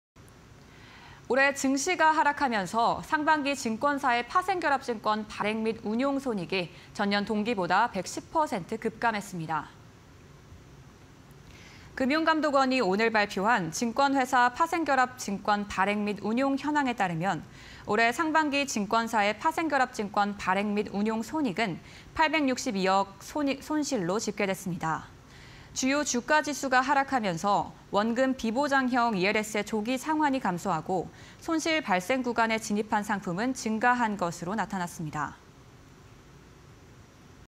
시장 침체에 상반기 증권사 파생결합증권 운용 손실 전환 (20221014)
올해 증시 하락 영향으로 상반기 증권사의 파생결합증권 발행 및 운용 손익이 전년 동기보다 110% 급감했습니다.

금융감독원이 오늘(14일) 발표한 '증권회사 파생결합증권 발행 및 운용 현황'에 따르면, 올해 상반기 증권사의 파생결합증권 발행·운용 손익은 862억원 손실로 집계됐습니다.

주요 주가지수가 하락하면서 원금비보장형 ELS의 조기 상환이 감소하고, 손실 발생 구간에 진입한 상품은 증가한 것으로 나타났습니다.